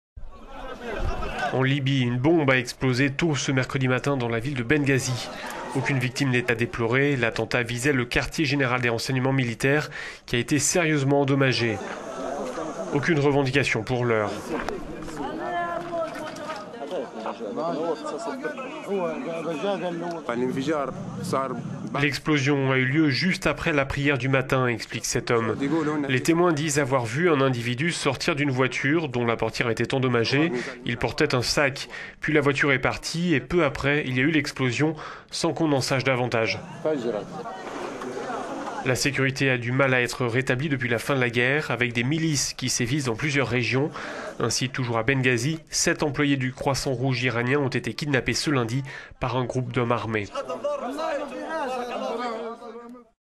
Libye: un bâtiment de la sécurité détruit par une bombe à Benghazi
En Libye, une bombe a explosé tôt ce mercredi matin dans la ville de Benghazi. Aucune victime n'est à déplorer. L'attentat visait le quartier général des renseignements militaires, qui a été sérieusement endommagé. Aucune revendication pour l'heure.

"L'explosion a eu lieu juste après la prière du matin, explique un des résidents de Benghazi. Les témoins disent avoir vu un individu sortir d'une voiture dont la portière était endommagée. Il portait un sac. Puis la voiture est partie, et peu après, il y a eu l'explosion, sans qu'on en sache davantage".

La sécurité a du mal a être rétablie depuis la fin de la guerre, avec des milices qui sévissent dans plusieurs régions. Ainsi, toujours à Benghazi, sept employés du Croissant-Rouge iranien ont été kidnappés ce lundi par un groupe d'hommes armés.